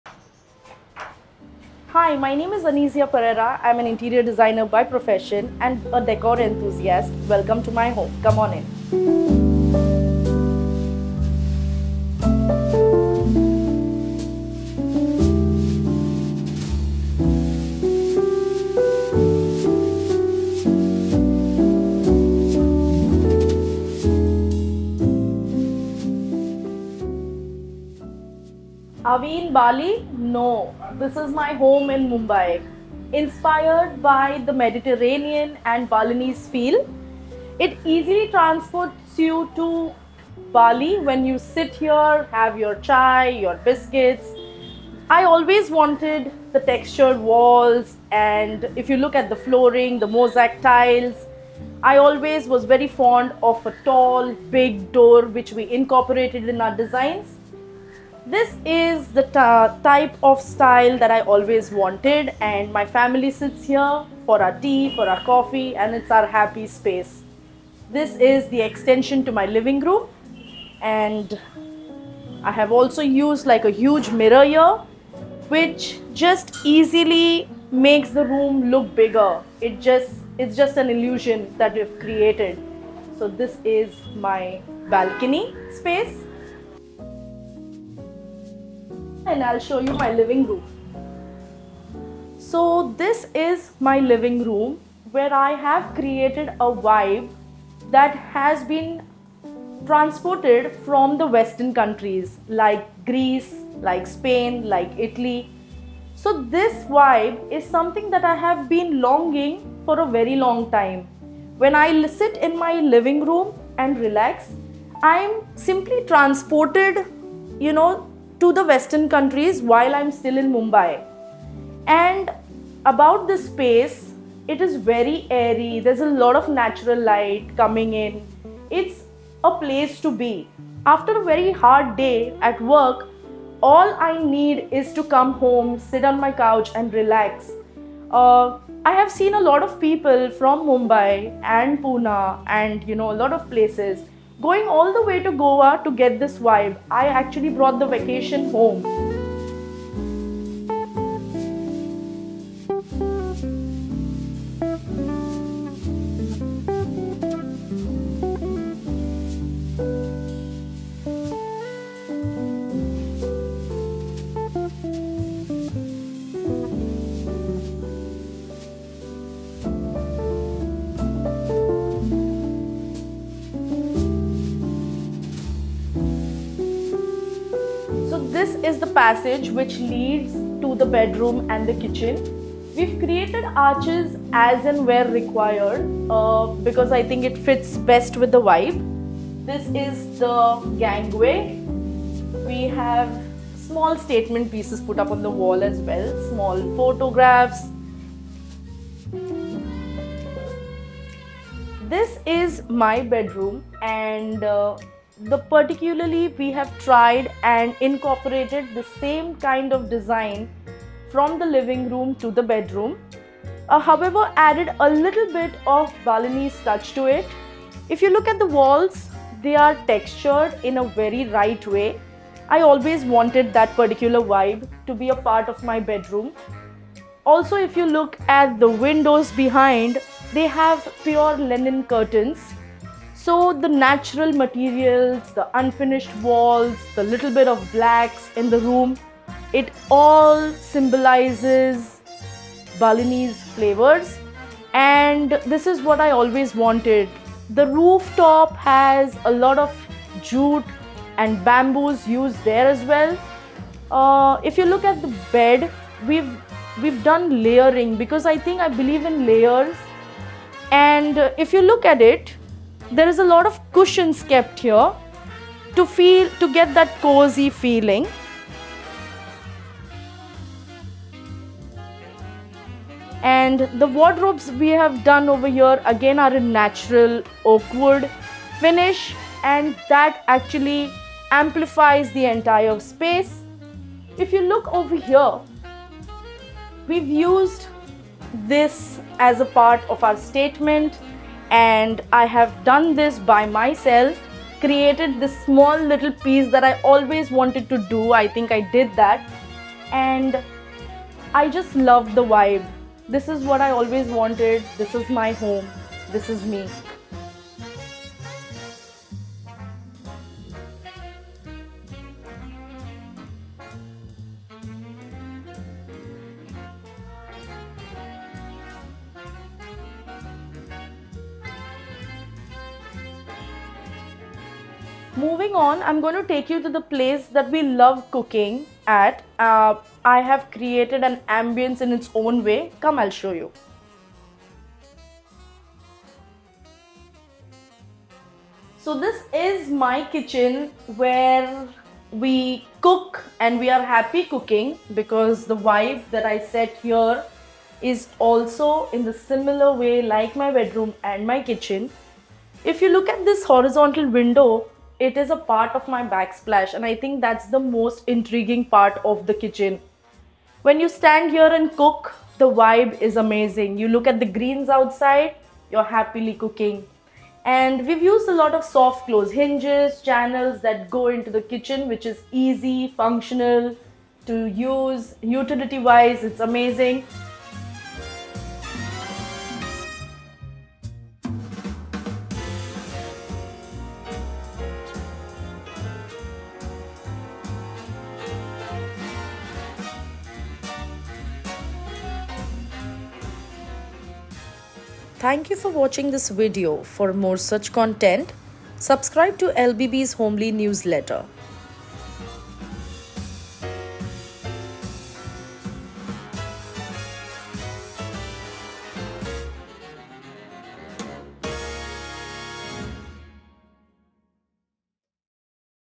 A Taste of Bali in Mumbai: Anizia's Mediterranean Inspired Home Tour
Step into Anizia's home in Mumbai, where the Mediterranean vibes transport you to a tropical paradise. From the moment you enter, you'll feel like you're on a holiday in Bali, surrounded by serene and calming elements. Join us as we explore the minimalist beauty of Anizia's room, featuring simple, clean lines that create a peaceful and relaxing atmosphere. Get inspired by her unique style and discover how to bring a touch of Bali into your own home.